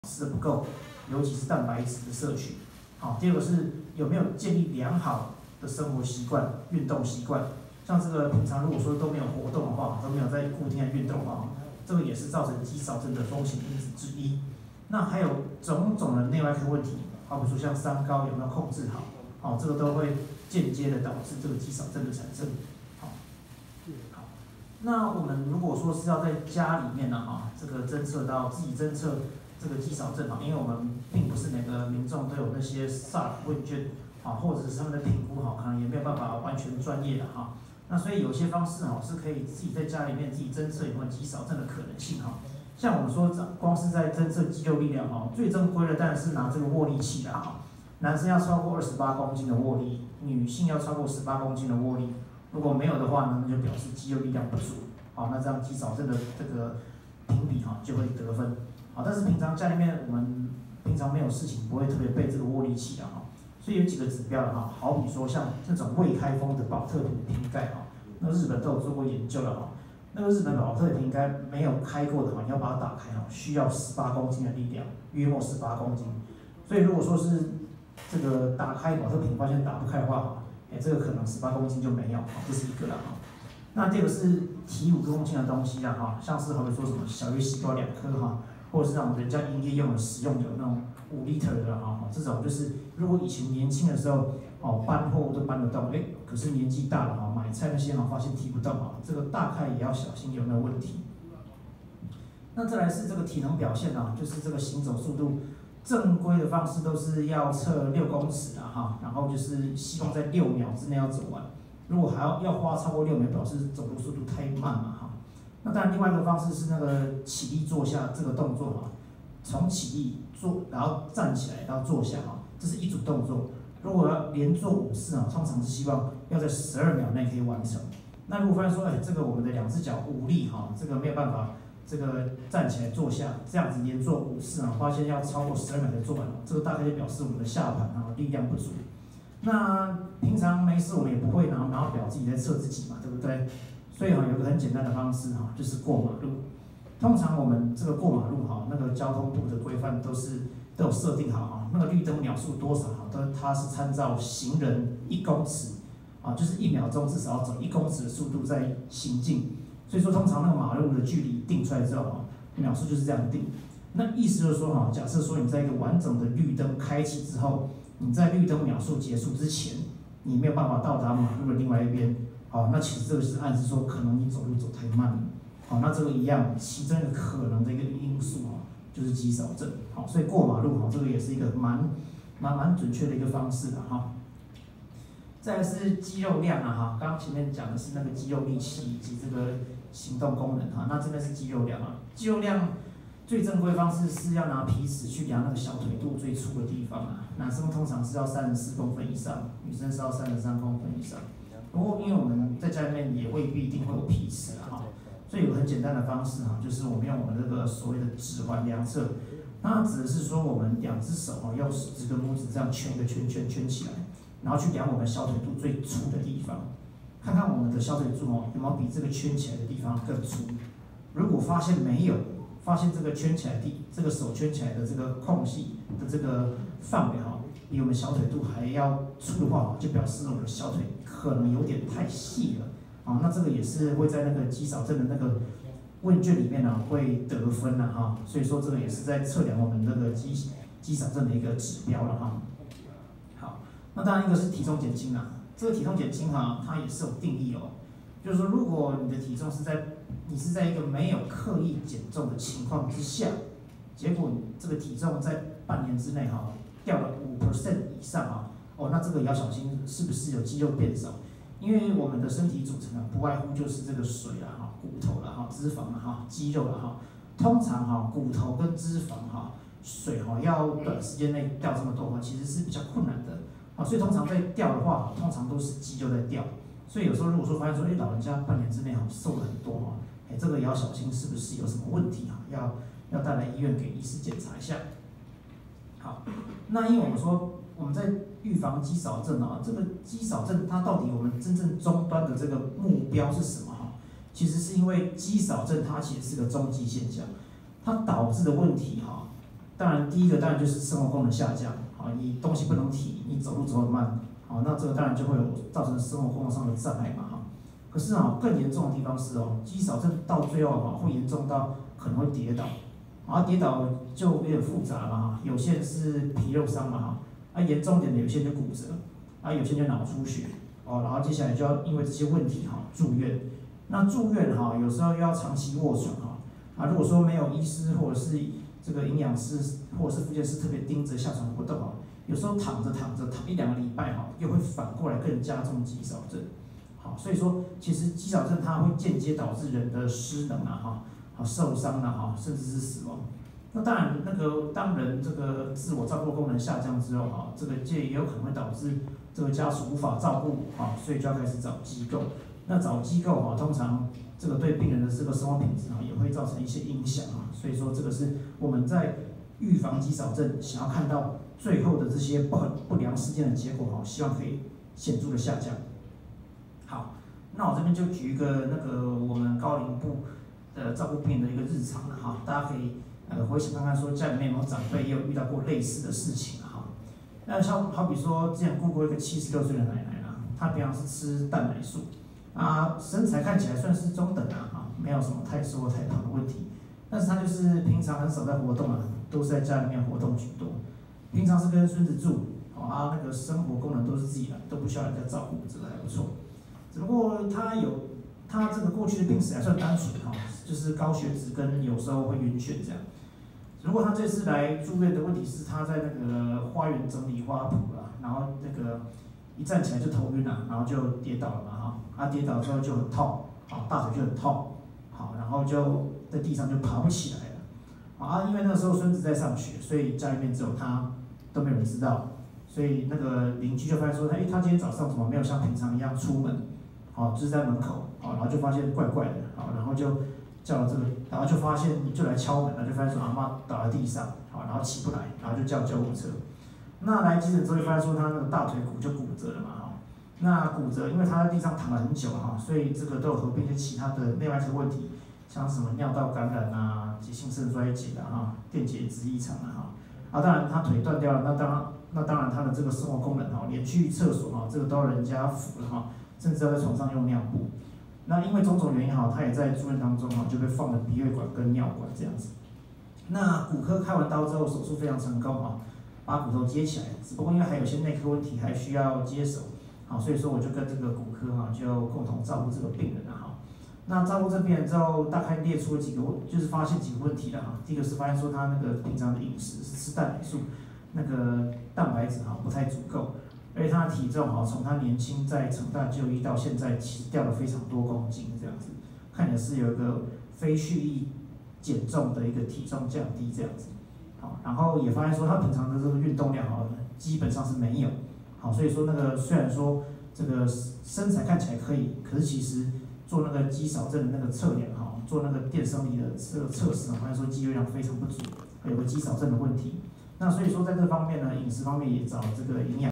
主講醫師簡報｜「高齡長者與肌少症」
姓名：黃議德
學歷：成功大學醫學系畢業
經歷：成大醫院神經部、高齡醫學部總醫師
現任：成大醫院高齡醫學部主治醫師
專長：老年醫學
神經退化疾病

高齡長者與肌少症

76歲的王奶奶與孫子同住，平時奉行蛋奶素飲食，身形中等，生活功能完全自理，但鮮少外出活動，有高血脂病史、容易暈眩。有次在整理盆栽時跌倒，導致骨折且無法自己站起來，所幸鄰居發現、叫救護車送醫。住院安排大腿骨骨折治療，也發現合併尿道感染、急性腎衰竭、電解質異常，導致急性功能衰弱，臥床20多天，有鼻胃管及尿管留置。在骨科手術後，與高齡醫學醫師共同照護。

成大醫院高齡醫學部黃議德醫師表示，患者經營養評估發現總熱量及蛋白質攝取量過少，日常運動習慣闕如，是肌少症的高風險族群。在適當止痛藥物控制疼痛後，物理治療師教導簡易術後復健運動，也指導在復原後應有之活動量，以保持肌肉體量、質量及功能。出院準備服務小組並申請長照2.0，協助餐食準備及洗澡服務。王奶奶返家後持續復健，不需輔具可自行行走，返回門診時進行生物電阻測量法(BIA)證實肌肉質量過低，經營養師衛教後已修正飲食習慣，並每天走路培養肌力。

臺灣於1994年已成為高齡化社會，2018年進階為高齡社會，將於2025年邁入超高齡社會；日本花了11年從高齡化社會發展成超高齡社會，臺灣卻只用了7年，老化速度在世界上數一數二。而肌少症的盛行率，是隨著年齡增加而更升高。

黃議德醫師提醒，要注意家中長者是否有鳥仔腳或走路變慢，俗話說：「樹老根先枯，人老腿先衰」，更要留意後續跌倒、臥床的風險。可先透過SARC-F問卷評估，並以能否在6秒之內走完6公尺的距離，或在12秒內連續5次起立、坐下，篩檢肌肉力量是否不足。

如疑似有肌少症，就應儘快尋求高齡醫學科、神經內科、復健科等專業人員評估、治療，避免、降低高齡長者因肌少症喪失日常生活功能，導致跌倒、受傷而住院，在住院中的長時間臥床又加重肌少症，形成惡性循環。成大醫院在2020年成立高齡照護中心，提供高齡患者完善的一站式服務。

黃議德醫師說明，至於日常預防肌少的方式，首先是每日飲食的總熱量要足夠，且要增加蛋白質的攝取，如奶蛋魚豆肉類。根據亞洲肌少症工作小組的指引，健康老年人每日蛋白質攝入量每公斤體重至少≥1.0g，肌少症或較衰弱者每日蛋白質攝入量則要提高到≥1.2g。而肌肉的生理維持也需避免維生素D不足，所以蕈菇類、魚類、蛋奶類的攝取，及一天陽光日照至少10分鐘都非常重要。同時要有運動的習慣，建議要混合有氧、阻力及柔軟度訓練。